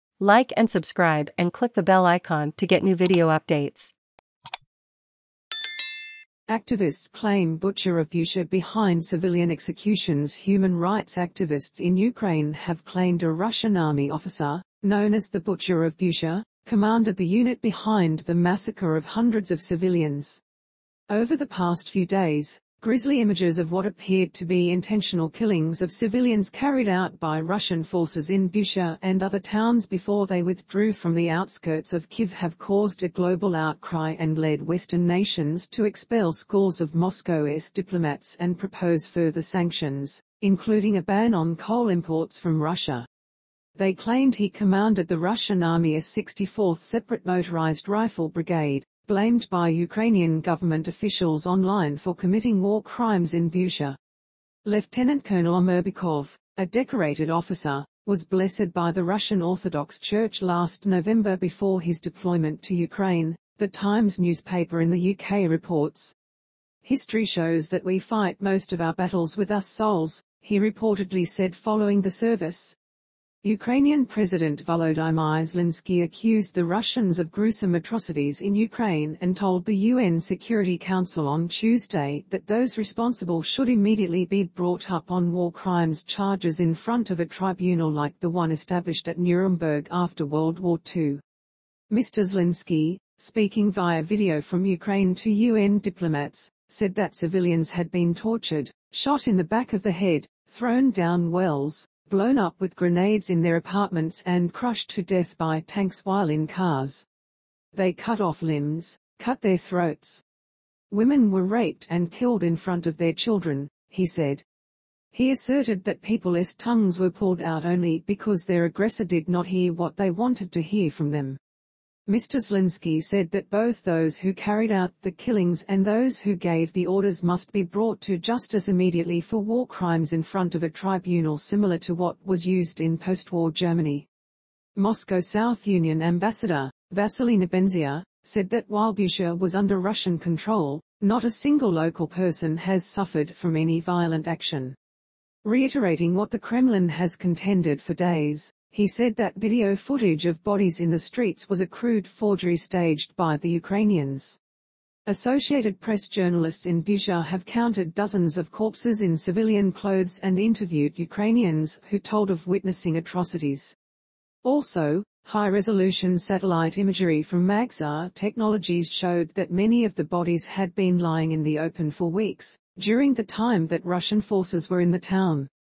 ACTIVISTS CLAIM 'BUTCHER OF BUCHA' BEHIND CIVILIAN EXECUTIONS
Human rights activists in Ukraine have claimed a Russian army officer. known as the 'Butcher of Bucha'. commanded the unit behind the massacre of hundreds of civilians. Over the past few days. grisly images of what appeared to be intentional killings of civilians carried out by Russian forces in Bucha and other towns before they withdrew from the outskirts of Kyiv have caused a global outcry and led Western nations to expel scores of Moscow's diplomats and propose further sanctions. including a ban on coal imports from Russia. They claimed he commanded the Russian army's 64th Separate Motorised Rifle Brigade. blamed by Ukrainian government officials online for 'committing war crimes' in Bucha. Lieutenant Colonel Omurbekov. a decorated officer. was blessed by the Russian Orthodox Church last November before his deployment to Ukraine. The Times newspaper in the UK reports. 'History shows that we fight most of our battles with our souls.' he reportedly said following the service. Ukrainian President Volodymyr Zelenskyy accused the Russians of gruesome atrocities in Ukraine and told the UN Security Council on Tuesday that those responsible should immediately be brought up on war crimes charges in front of a tribunal like the one established at Nuremberg after World War II. Mr Zelenskyy. speaking via video from Ukraine to UN diplomats. said that civilians had been tortured. shot in the back of the head. thrown down wells. blown up with grenades in their apartments and crushed to death by tanks while in cars. 'They cut off limbs. cut their throats. Women were raped and killed in front of their children.' he said. He asserted that people's tongues were pulled out 'only because their aggressor did not hear what they wanted to hear from them'. Mr Zelenskyy said that both those who carried out the killings and those who gave the orders 'must be brought to justice immediately for war crimes' in front of a tribunal similar to what was used in postwar Germany. Moscow's UN ambassador. Vassily Nebenzia. said that while Bucha was under Russian control. 'not a single local person has suffered from any violent action.' Reiterating what the Kremlin has contended for days. he said that video footage of bodies in the streets was 'a crude forgery' staged by the Ukrainians. Associated Press journalists in Bucha have counted dozens of corpses in civilian clothes and interviewed Ukrainians who told of witnessing atrocities. Also. high resolution satellite imagery from Maxar Technologies showed that many of the bodies had been lying in the open for weeks. during the time that Russian forces were in the town.